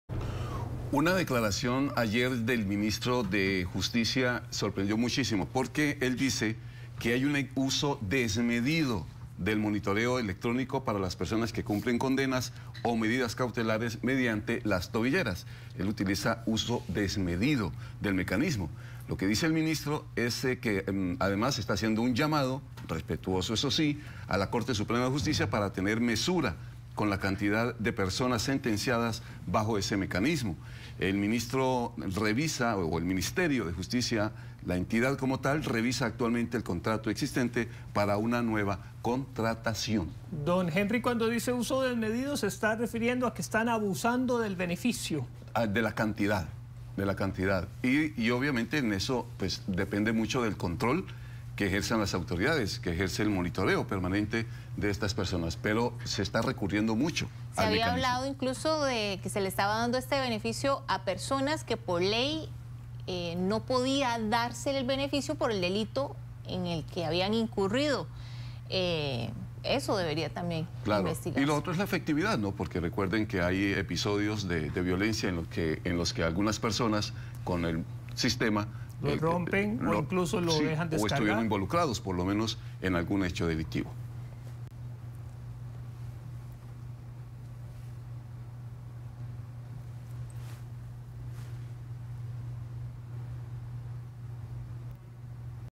"Uso desmedido" del monitoreo electrónico
Ministro de justicia solicita mesura con sentencias.